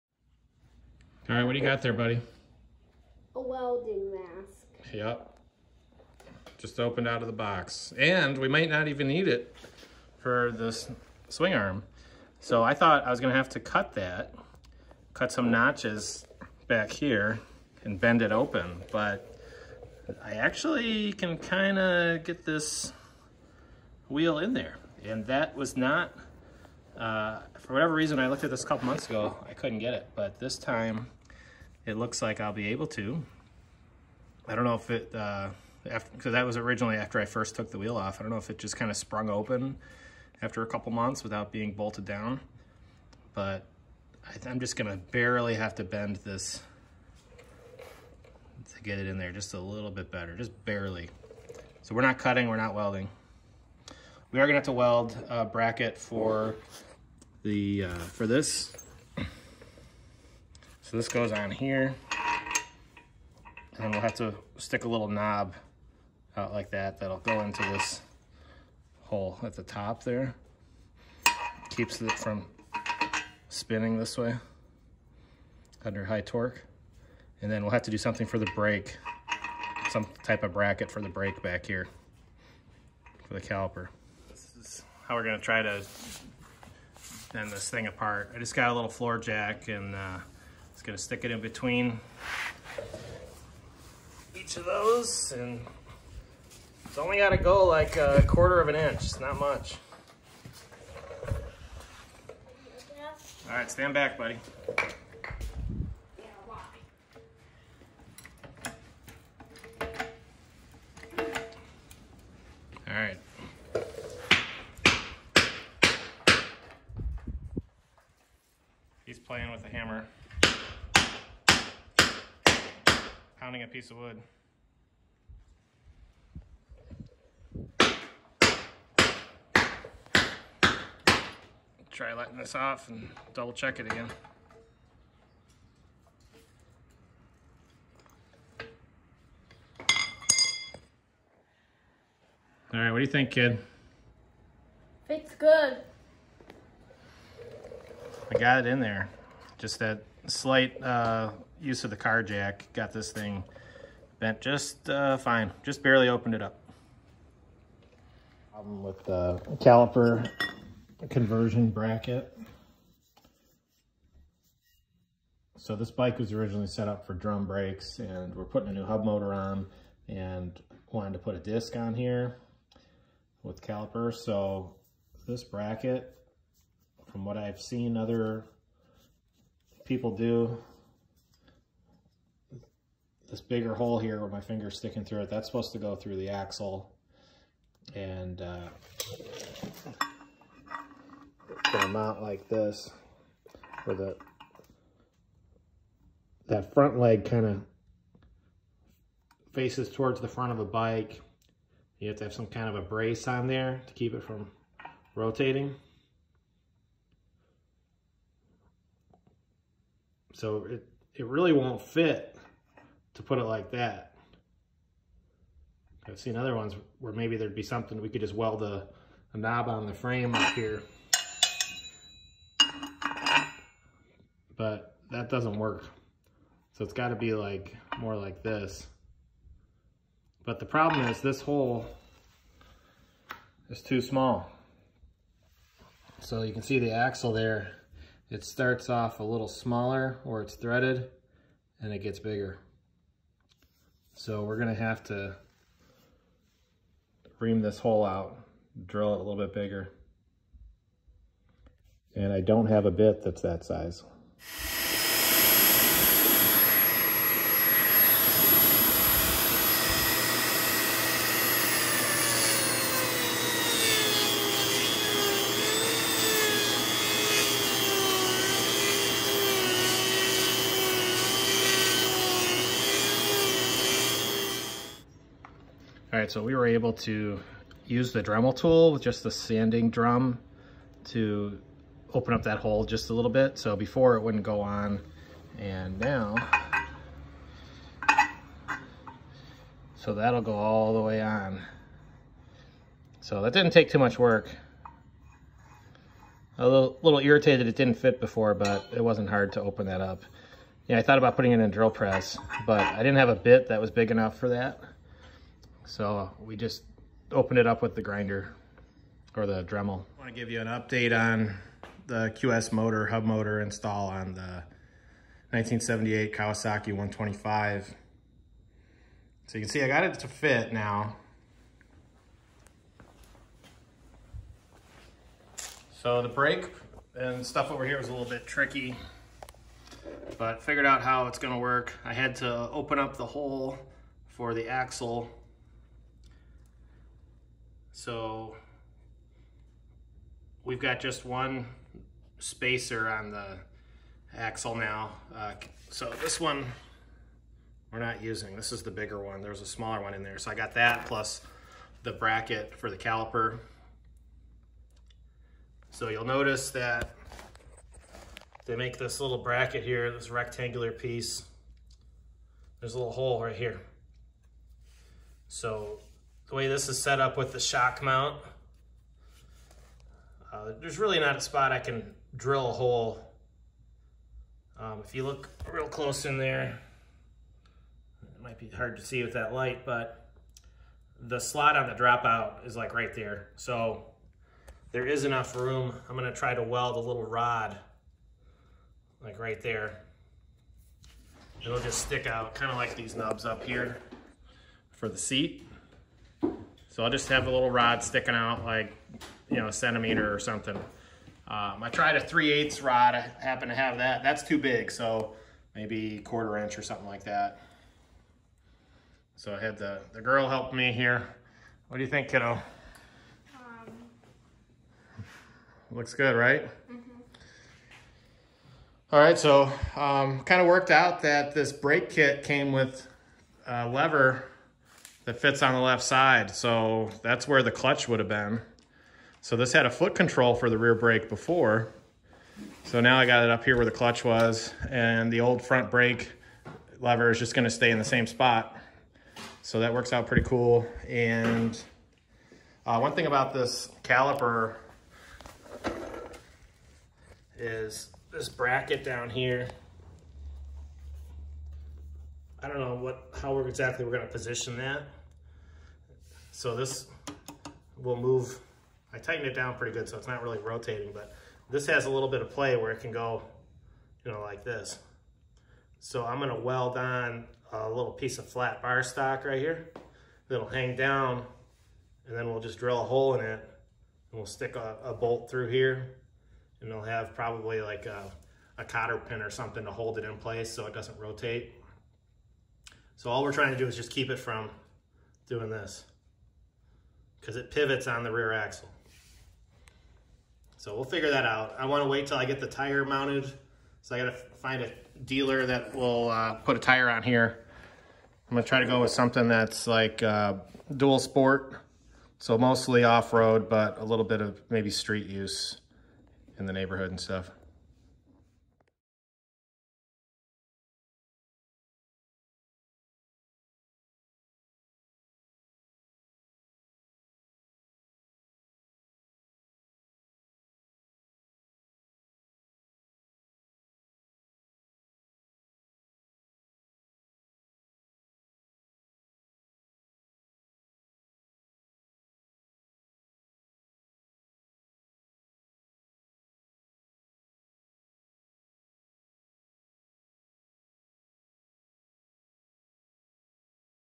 Hub motor and disc brake mounting
Mounting the QS Motors electric hub motor and disc brake kit on 1978 Kawasaki 125. The hub motor is a 3.5x17, 4000w 273 40H V3 cover C options with QS Motors.